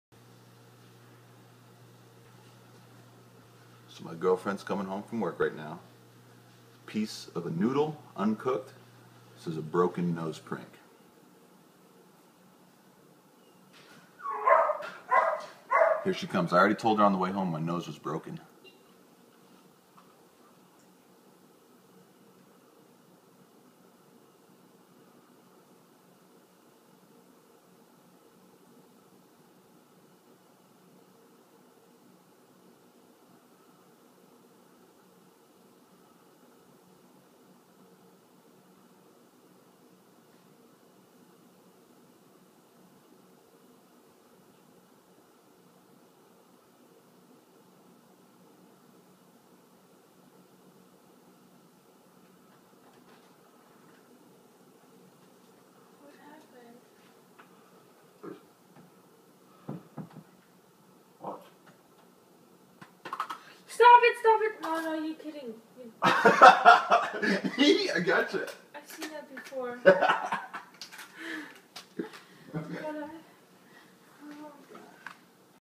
Broken Nose Prank
The first of many to come! She'd seen this one before, so the reaction was minimal, although still good.